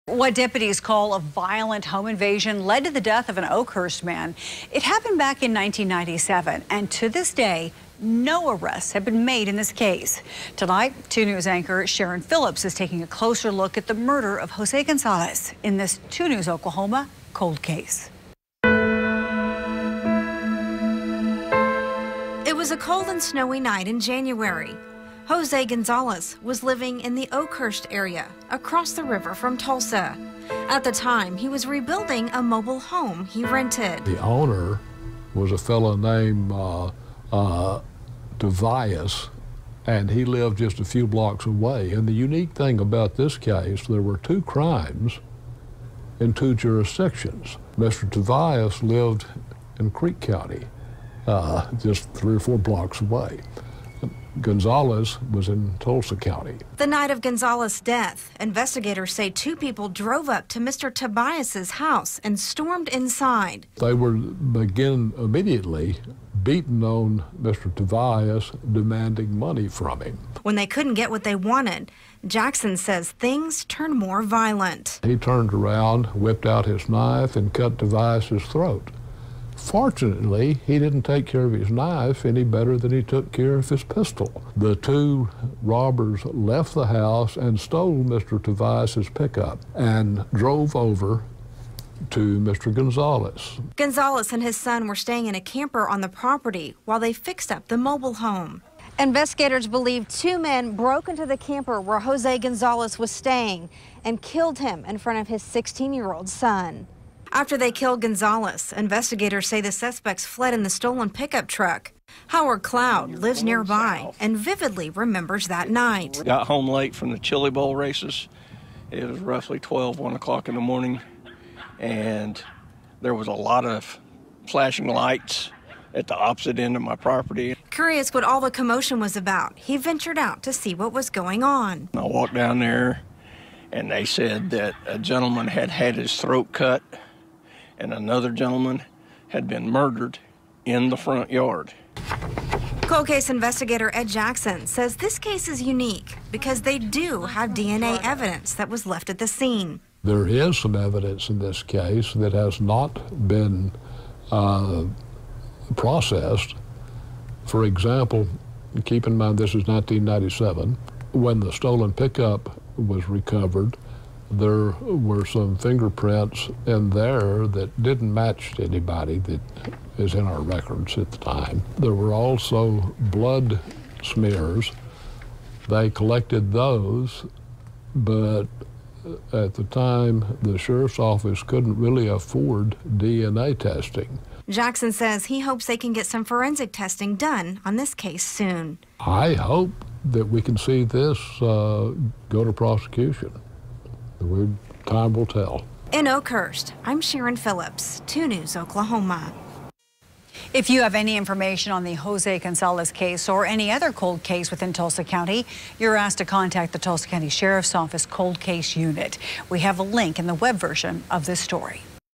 Oklahoma's Cold Case Files: The Death of Jose Gonzales
The night of Gonzales' death, two people drove up to Mr. Tobias' house and stormed inside.

"They began immediately beating on Mr. Tobias, demanding money from him," said Jackson.

When they couldn't get what they wanted, Jackson says things turned more violent.

2 News' Sharon Phillips reports.